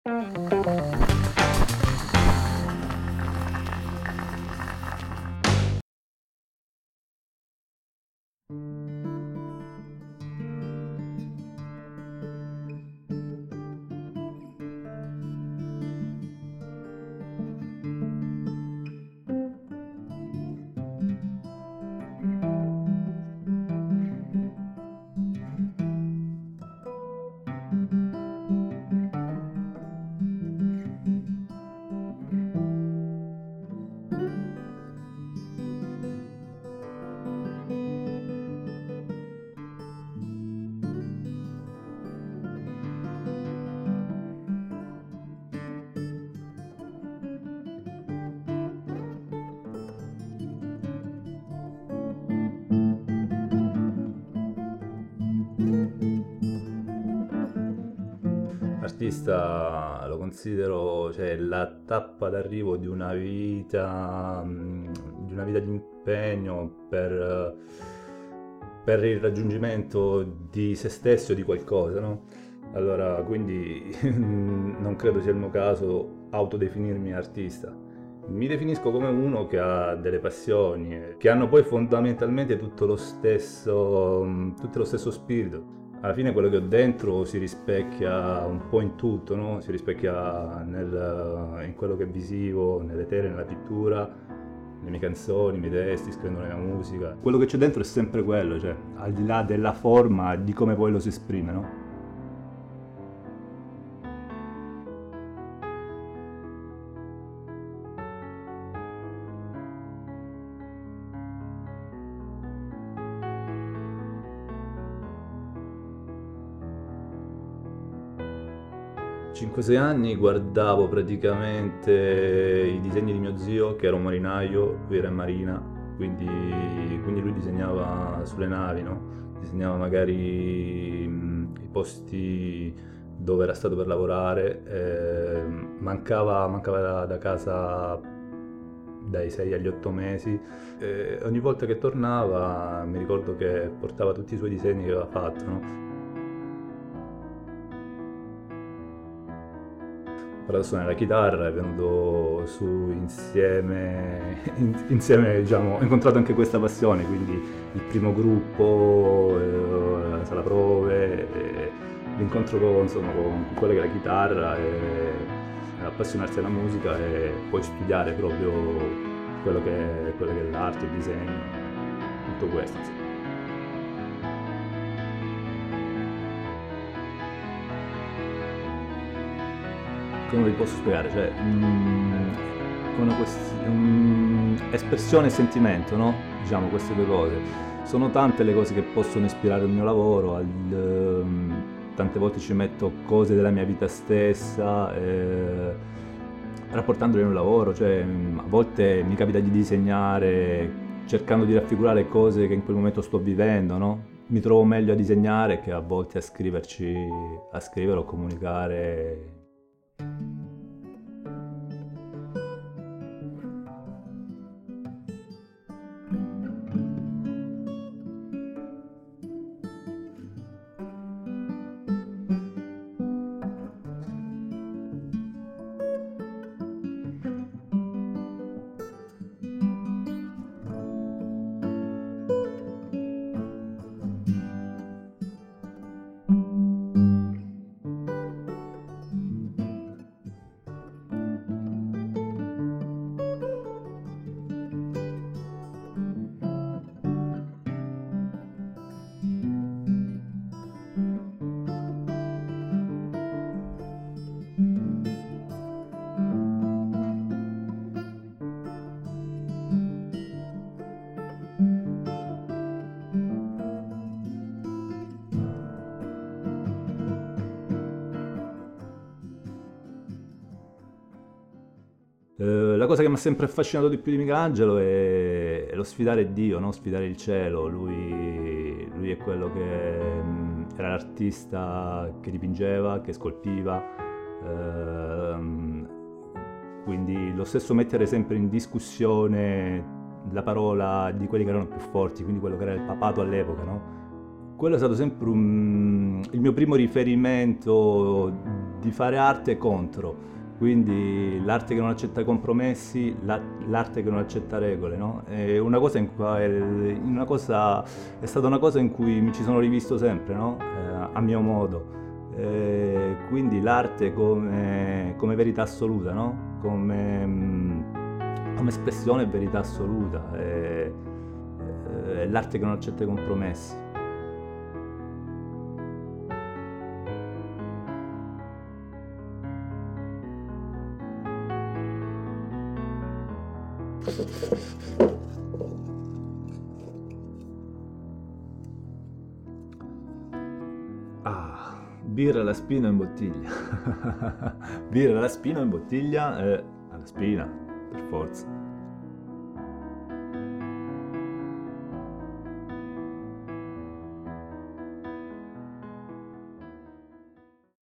Giuseppe Fracasso - live at Caffettino
Giuseppe Fracasso ci è venuto a parlare di come sono nate le sue passioni e di come sono evolute nel tempo...chitarra, pittura, disegno e...